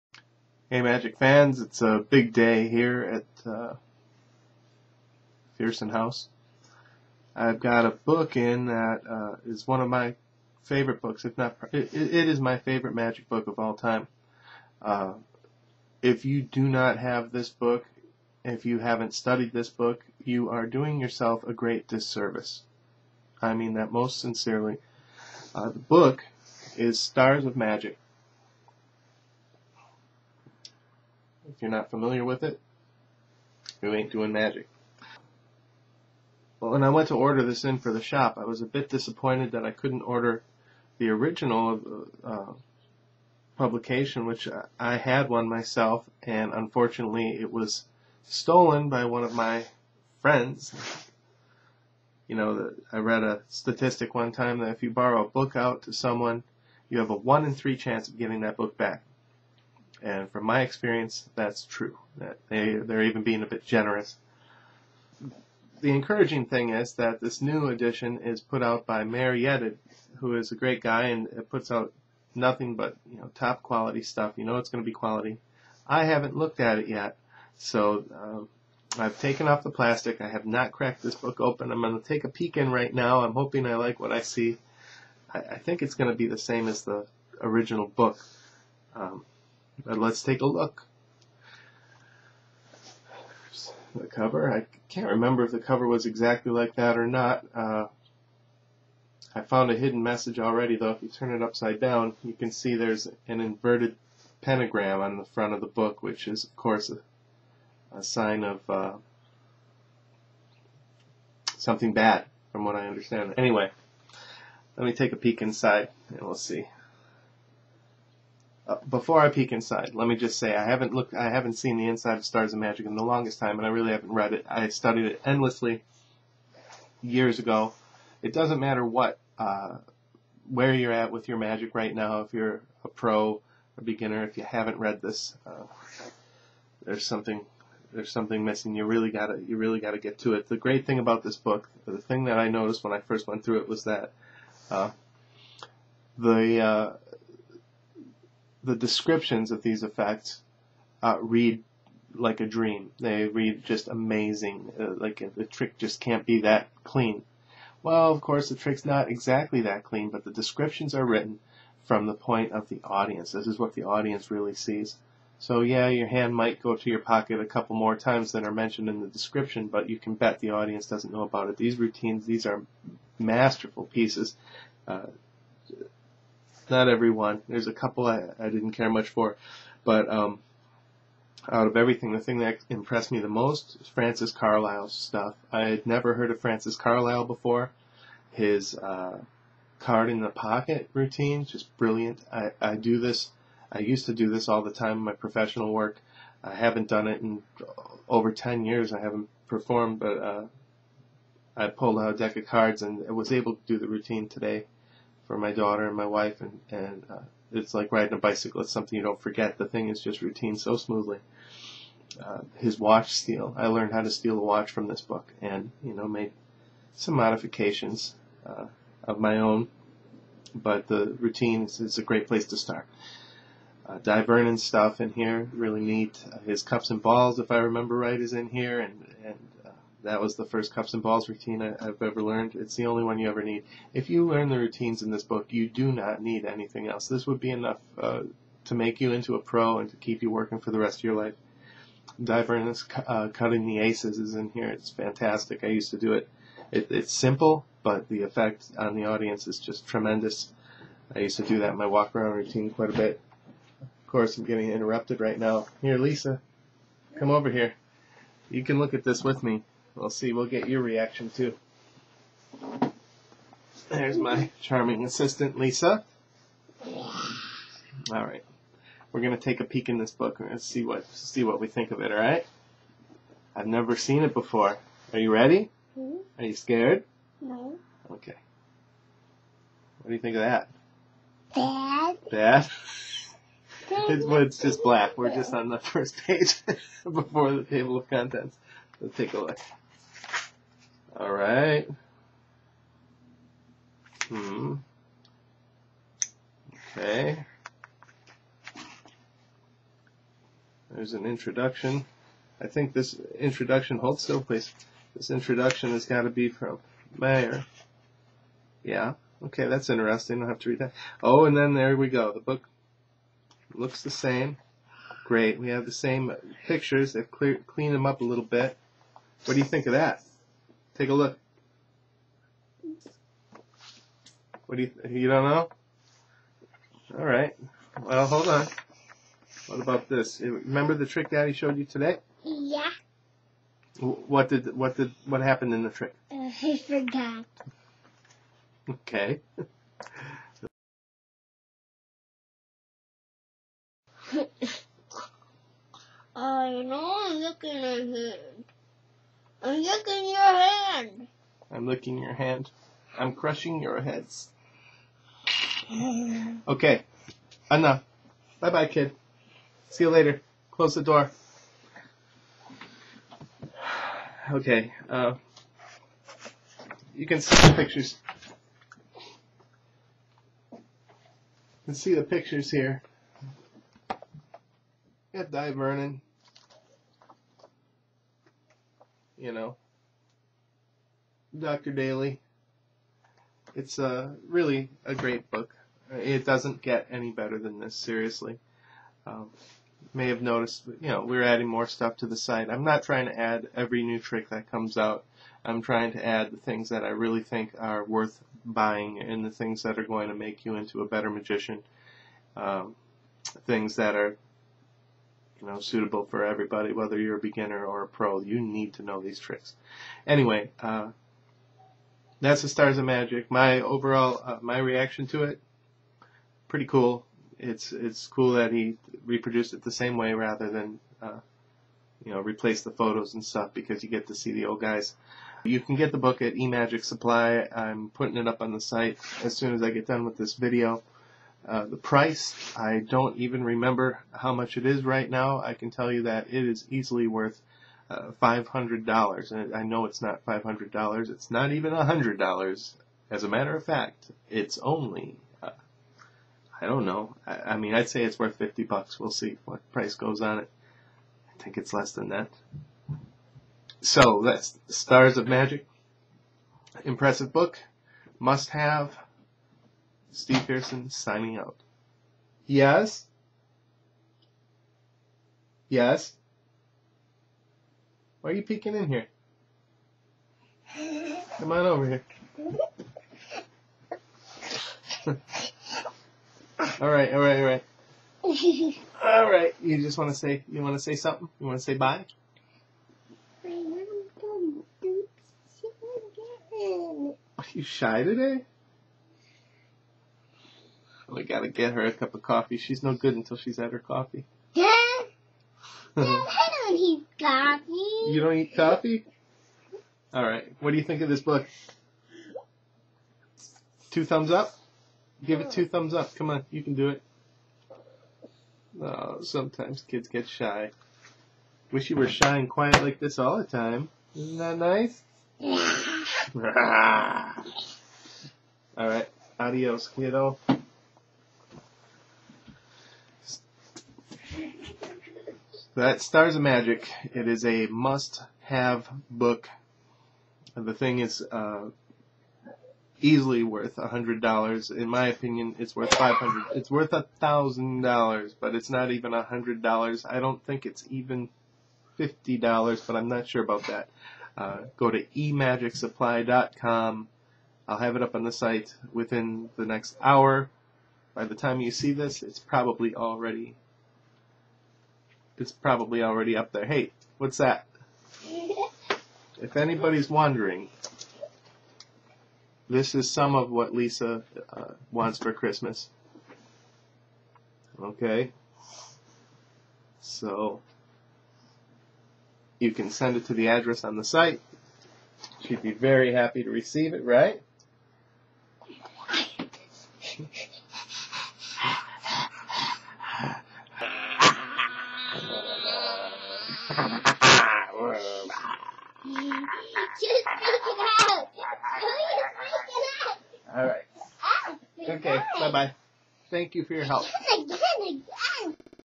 Stars of Magic - Reaction Video
I never saw this book before... at least the new edition.

Watch my reaction!